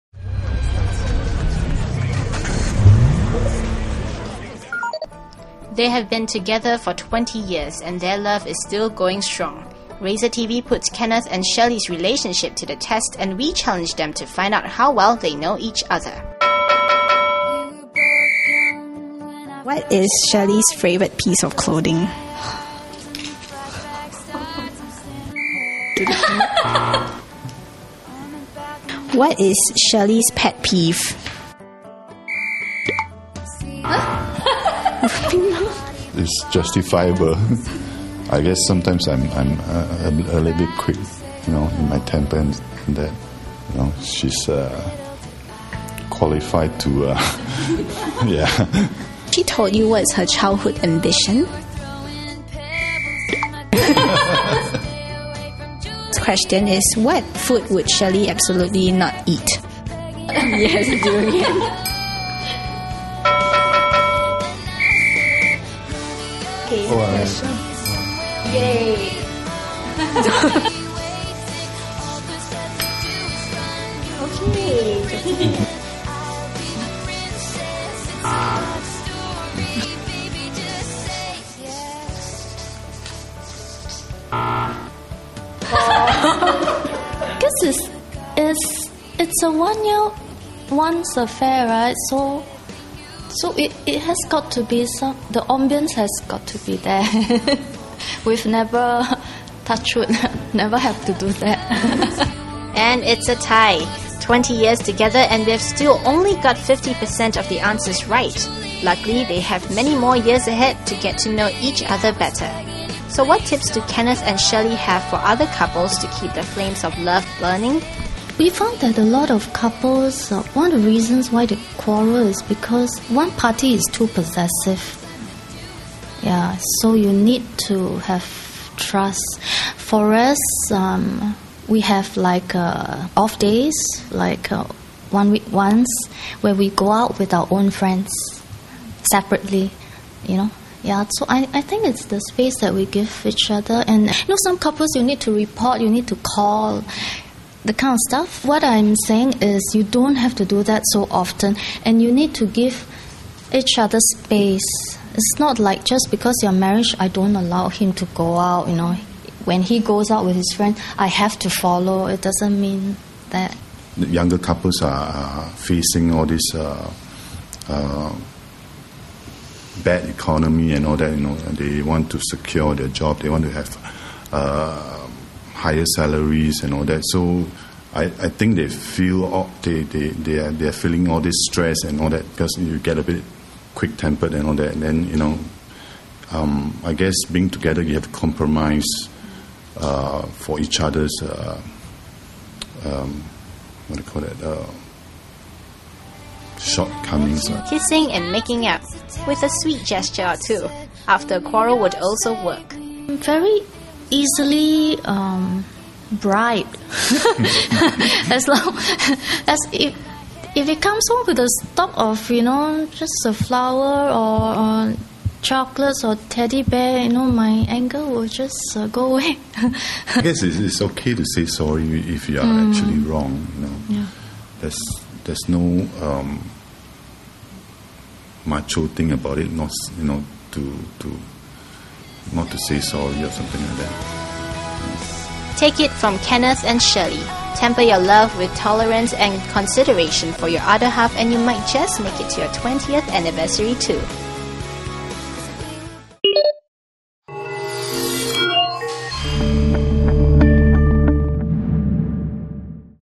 Did they fail the love test? (Love Story 1.4)
Shirley and Kenneth have been together for two decades, but how well do they know each other?Let's find out!